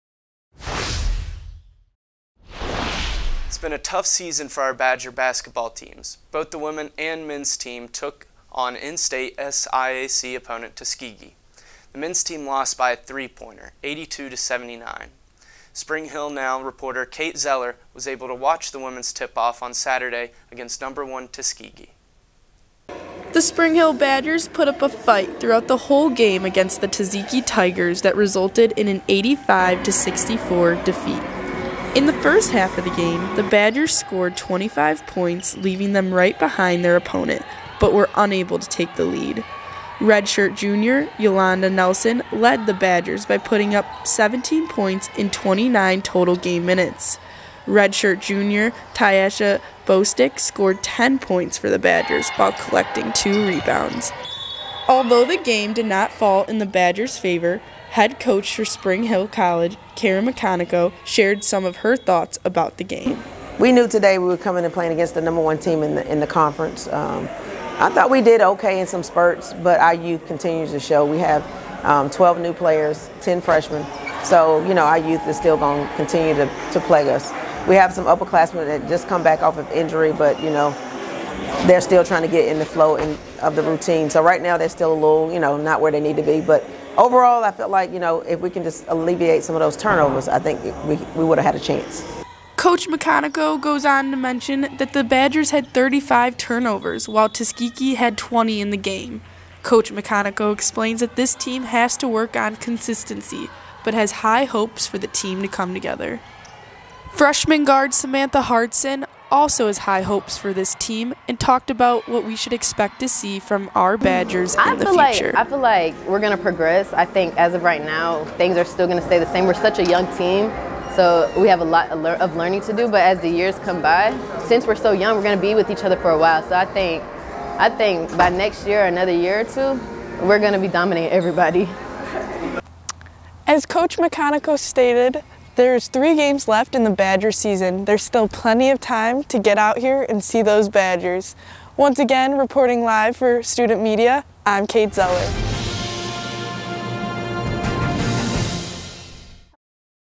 SHC vs. Tuskegee Women's Basketball highlights
SHC sports anchor Nic Watts introduces coverage of the women's basketball game against Tuskegee.